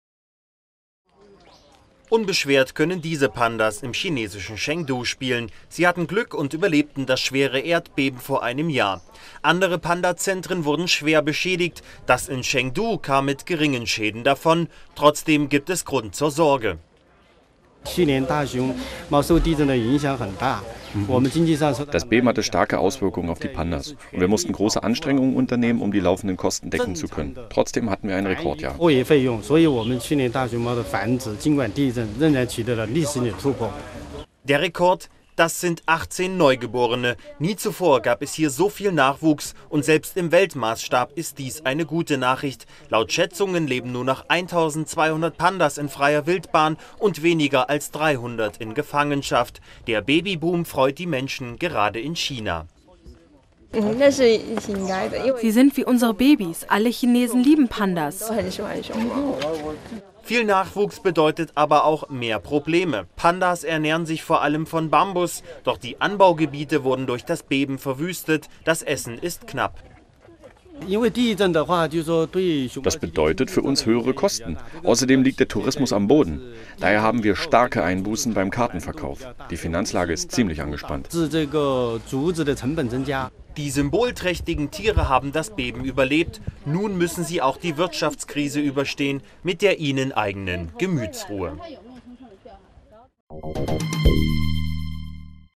Pandazentren in China leiden noch unter Beben vor einem Jahr
Ein Jahr nach dem verheerenden Erdbeben in der chinesischen Provinz Sichuan leiden die Pandazuchtstationen in der Region weiter unter den Folgen. Viele von ihnen wurden stark zerstört, andere wie die Forschungsstation Chengdu kämpfen mit Futterknappheit, weil das Beben weite Teile der Bambusflächen verwüstet hat. Und weil weniger Touristen kommen, gehen auch die Einnahmen aus dem Verkauf von Eintrittskarten zurück.